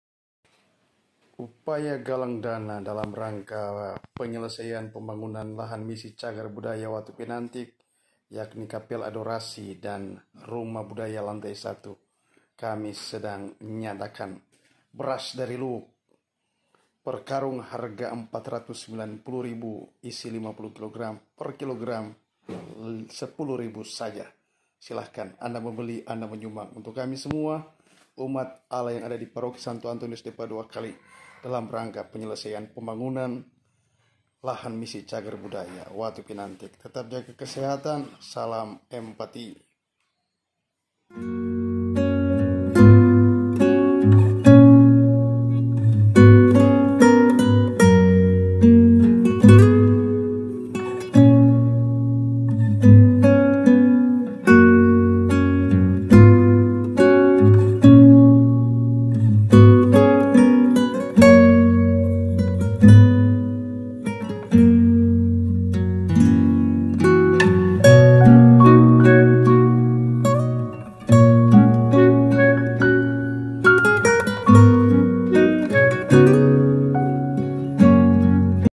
UPAYA GALANG DANA
Dijual beras dari Luwuk, 1 karung isi 50 kg harga Rp  490.000 dan per kg Rp. 10.000; MEMBELI = MENYUMBANG.
Terbatas hanya 20 karung persediaan, di Pastoran Gereja Katolik Lotta, Jaga 2, Kec. Pineleng.